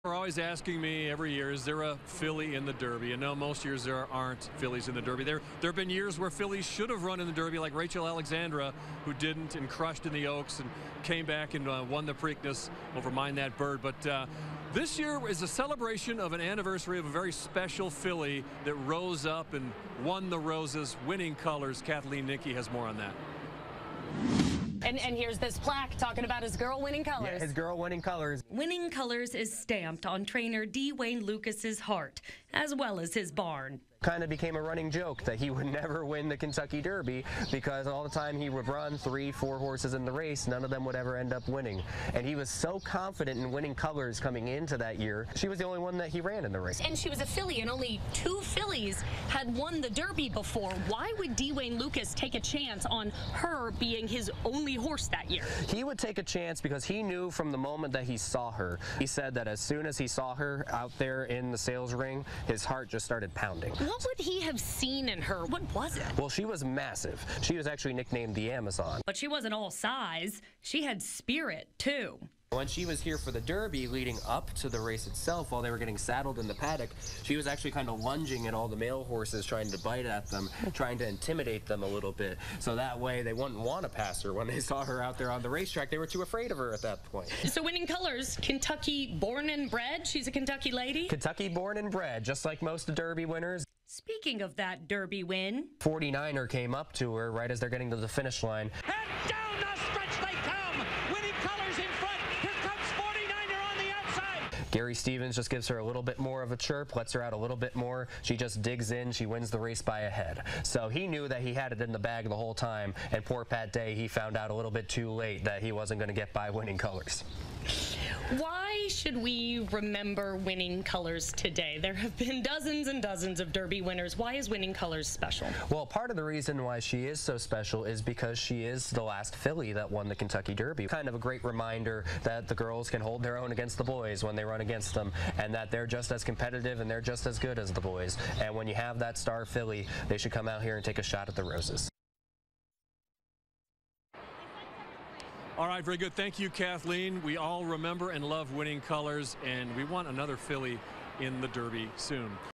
35 years later, remembering Winning Colors as the last filly to win the Derby
Winning Colors remains one of just three fillies ever to win the Kentucky Derby.